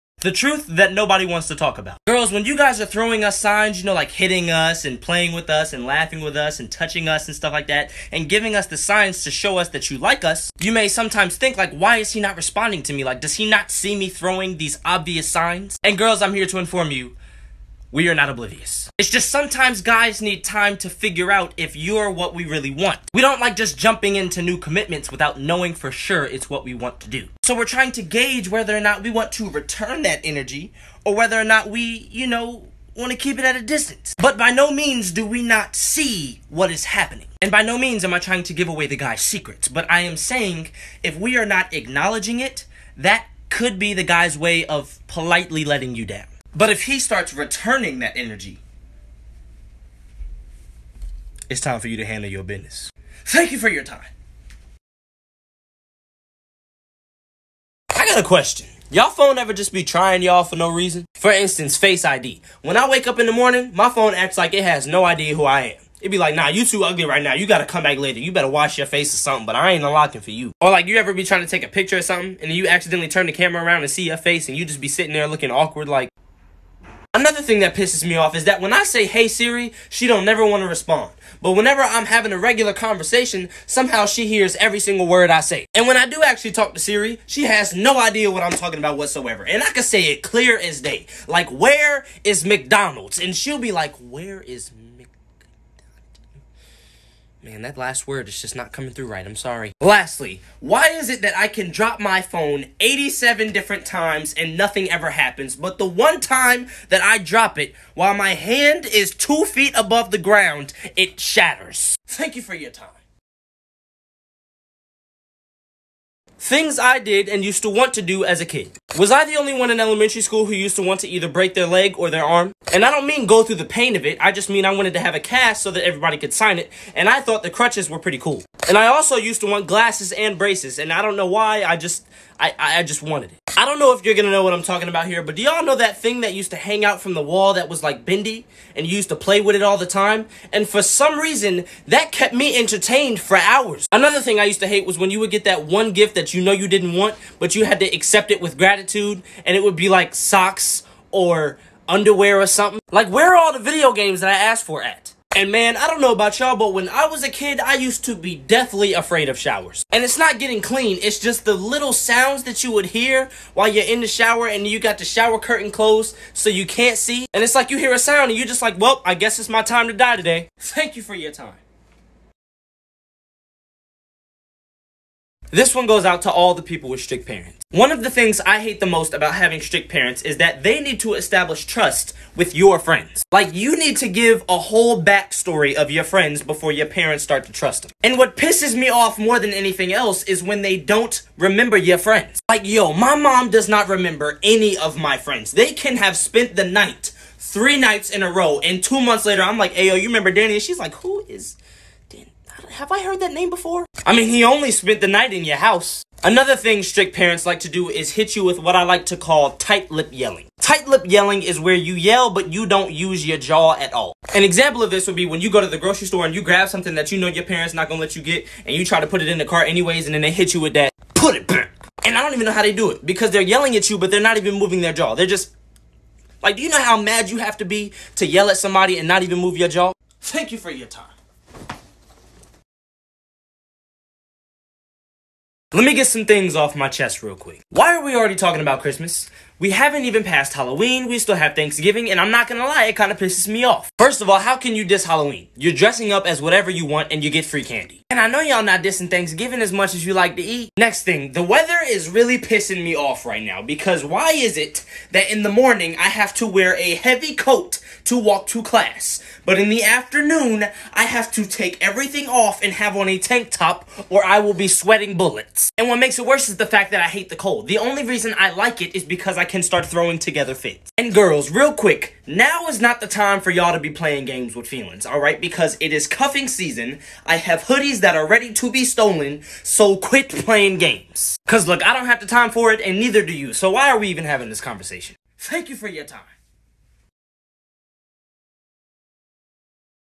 Xavier Henry: " Thank you for your time!" Tik Tok Compilation.
Like I said lol, I always have different ideas for these video compilations. Maybe some other people have done it, but I just want to be part of the trend.

Anyways, here's XAVIER HENRY'S  " Thank you for you time! " Compilation. I'll try to make my videos longer in the future!.

Xavier Henry is one of my favorite Tik Tokkers, ever since I started using the app, from my other account, his one minute videos were enough to get me through a anger mood.